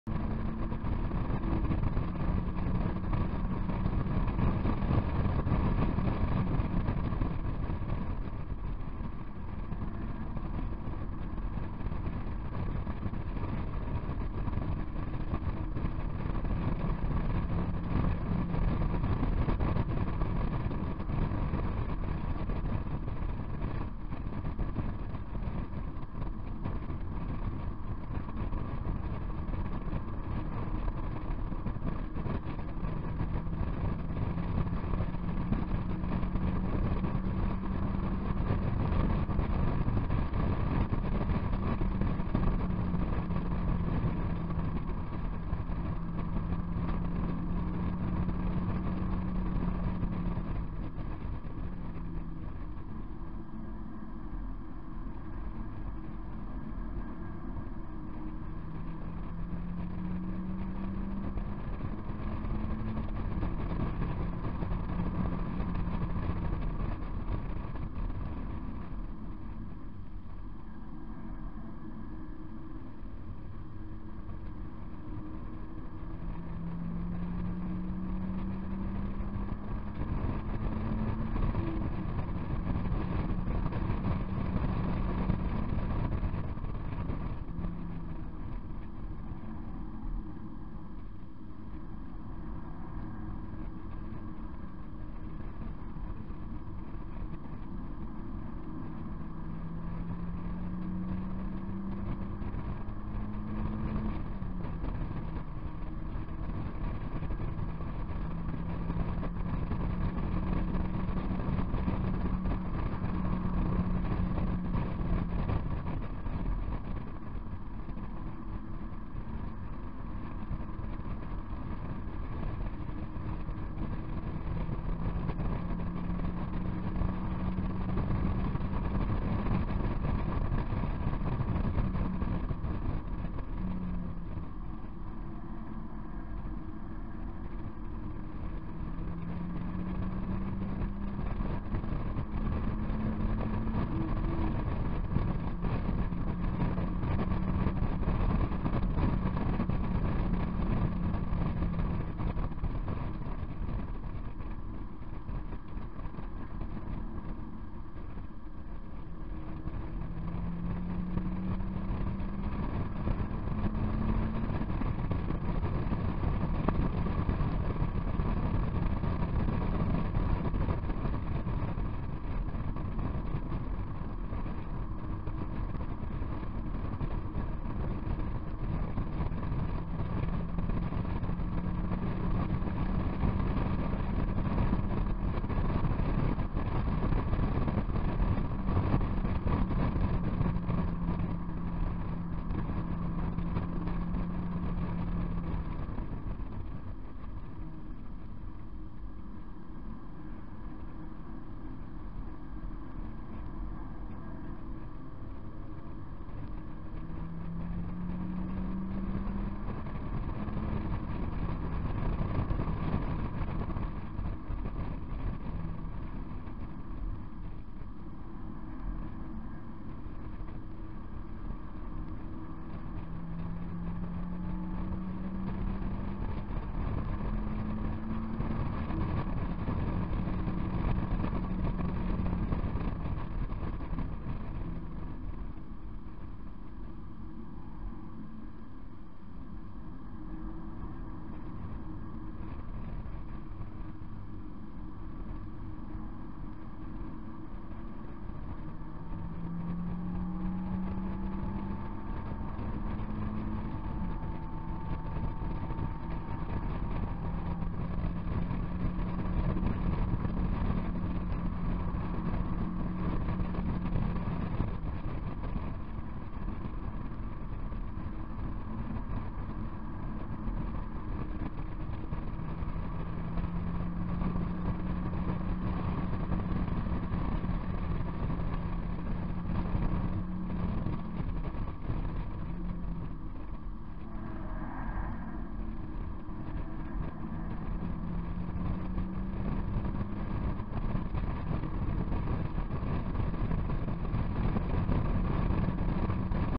MX 5 Oulton Park 18 Nov 2016
On board lap of NB MX-5 at Oulton Park track day run by 'Mazda on Track'